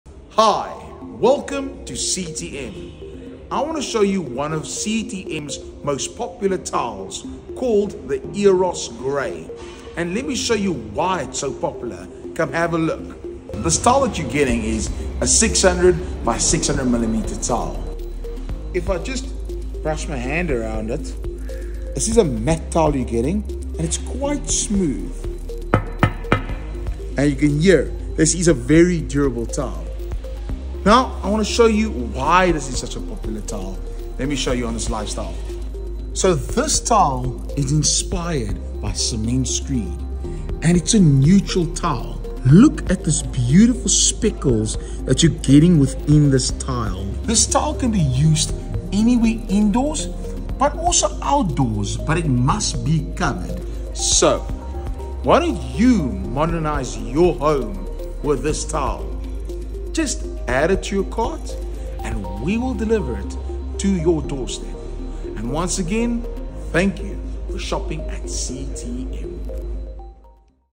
How does it look & feel? Eros Grey EcoTec Matt Porcelain Floor Tile
Grey tiles come in all shapes & sizes; Damien explains why the Eros Grey Tile will modernize your home. He shows us how realistic this large format tile is and tells us where this tile can be used.